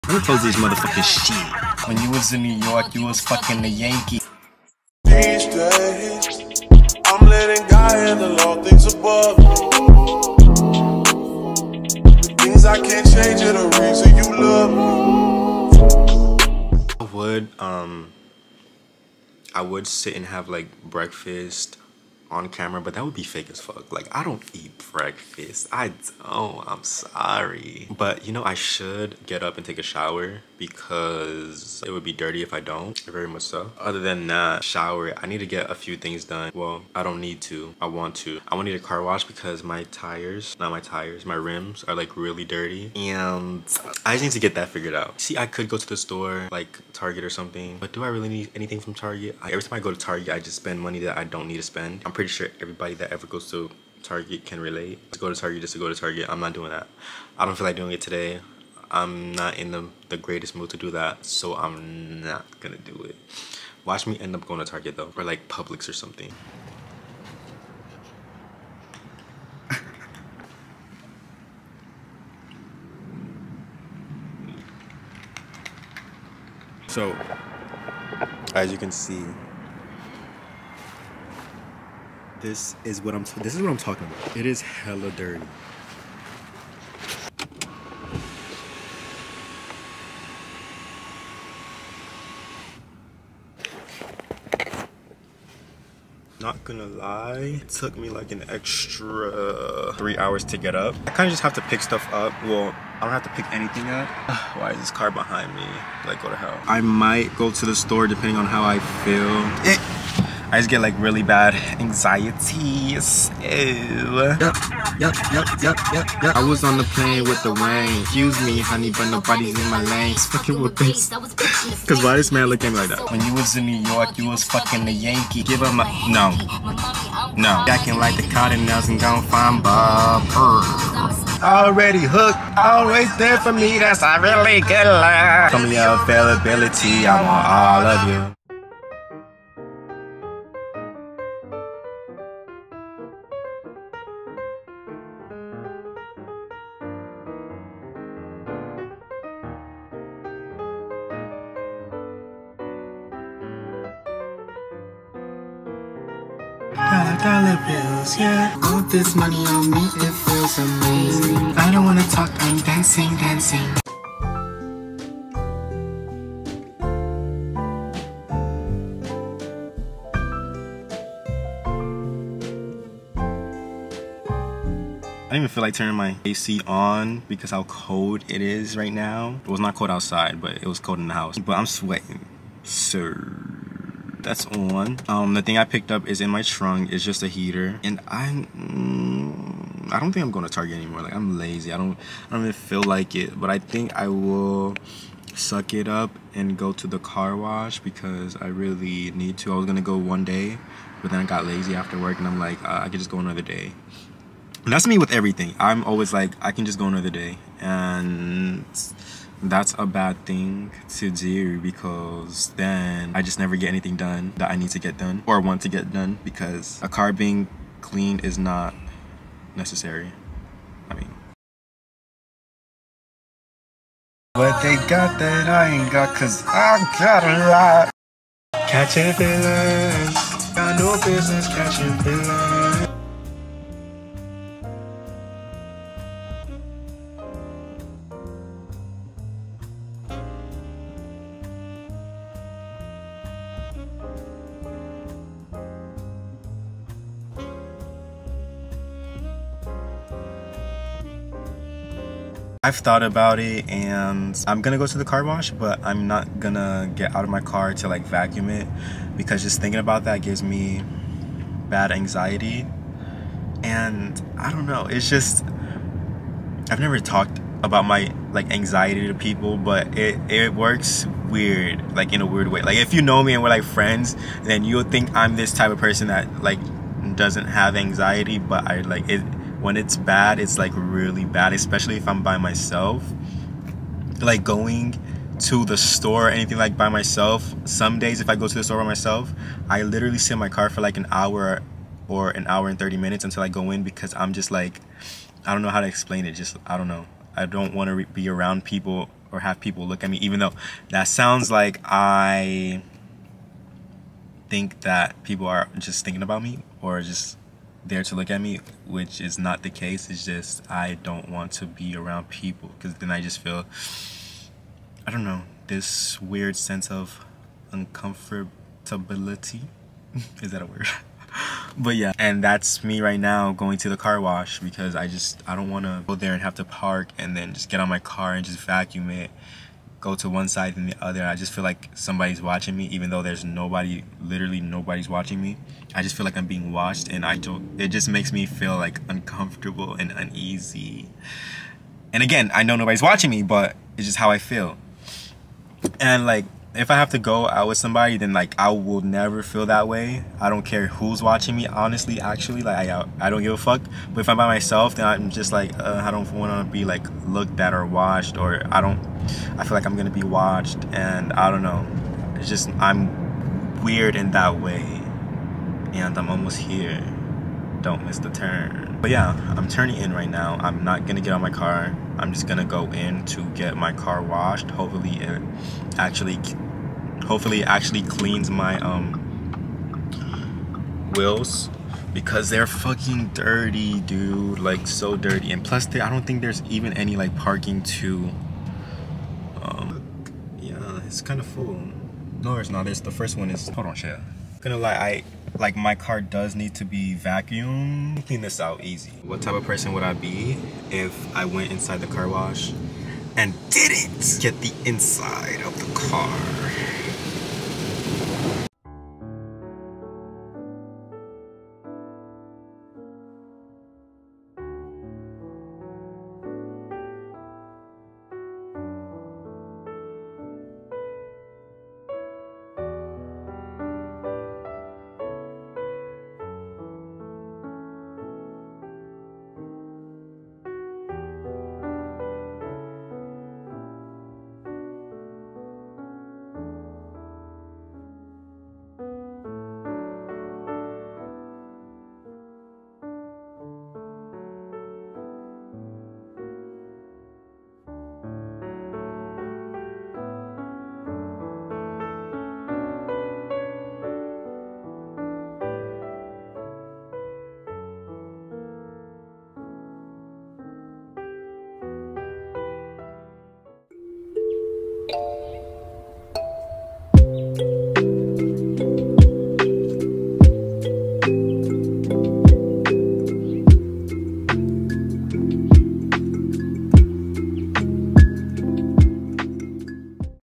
condensing my day into 8 minutes
condensing my day into 8 minutes - showing you guys what my day consisted of in eight minutes. (pretty much 7 not 8 if you don’t count the intro & outro) LMAO

waking up, singing in car- bootleg karaoke, talking about my anxiety, going to car wash.